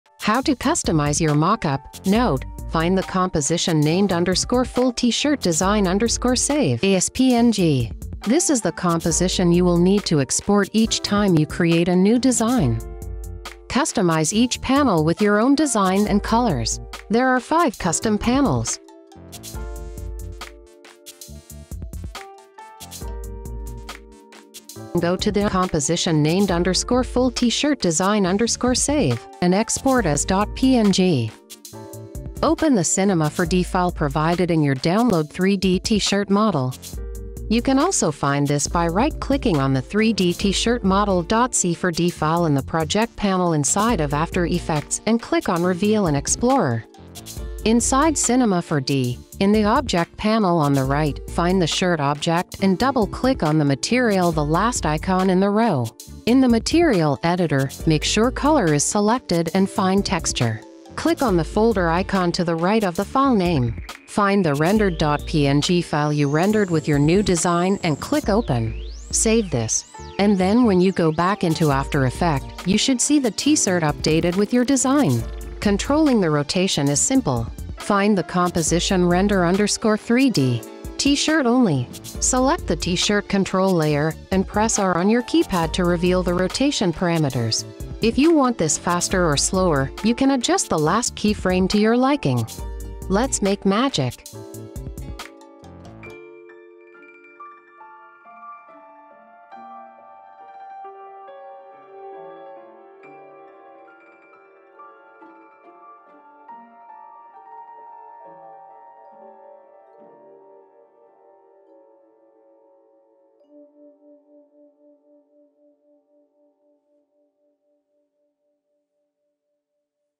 3D Rotating T-Shirt Mockup Template for AFTER EFFECTS
Transform your brand with our fully customizable 3D T-shirt mockup template in MINUTES—exclusively in After Effects! We will be using Cinema 4D Lite to replace the textures, but it comes free with After Effects.

CUSTOMIZE every detail: Sleeves, Front, Back, and Collar with your own designs and colors. Add your unique background for a standout look. Create stunning, realistic mockups in minutes with After Effects. Elevate your brand and make a lasting impression!

I would love to see the Magic you create with this! Tag me on Instagram if you post your creation @Anastasia_MakinMagic

Let's Make Magic!